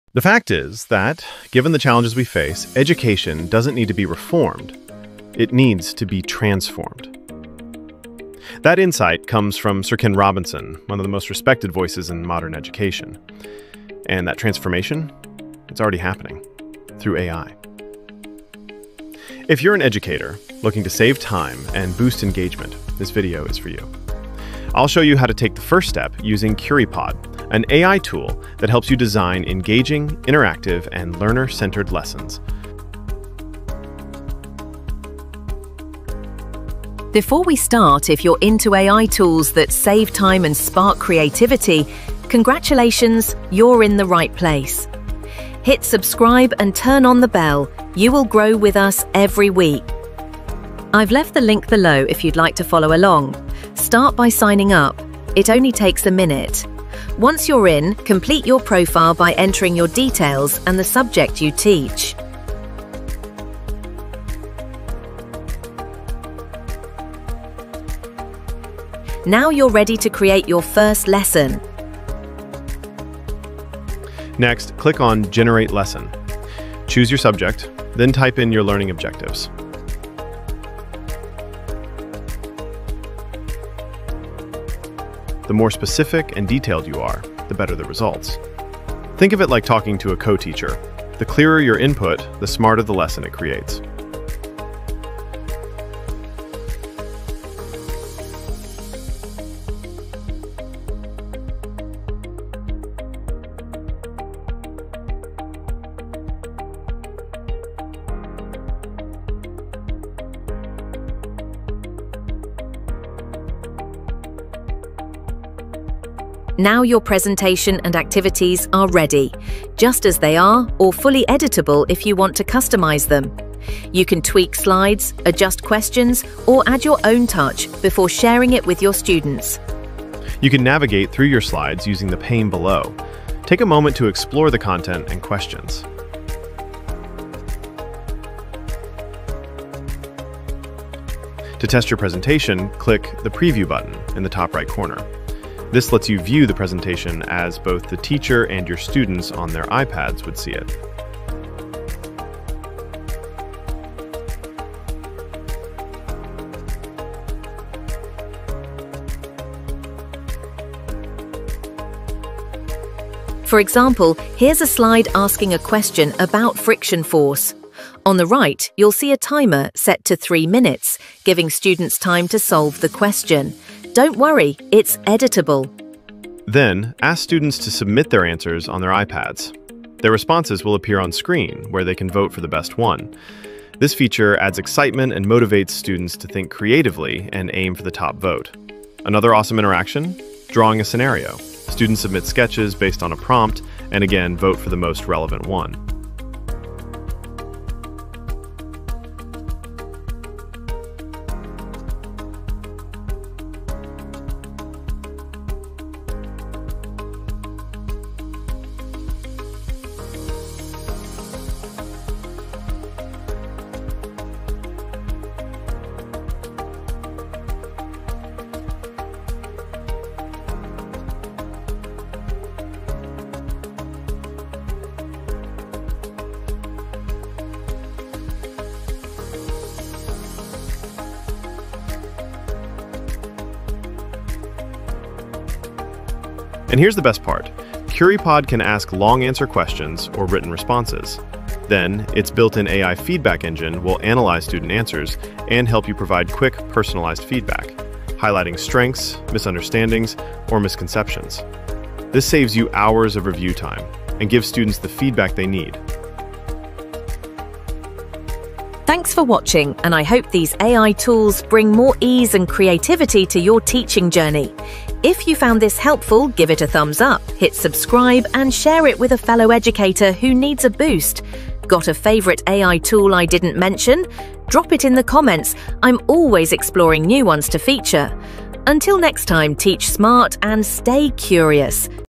Curipod Tutorial & Demo: AI Tool for Interactive Lesson Creation
In this video, I walk you through a full tutorial and demo of Curipod — an AI-powered tool designed to help educators create interactive, engaging lessons in minutes.

Whether you're teaching science, math, or language, Curipod lets you generate activities like:
✅ Think-pair-share questions
✅ Live polls and word clouds
✅ Auto-feedback on student answers
✅ Editable AI-generated presentations

🎯 Perfect for busy teachers who want to boost student engagement while saving valuable time.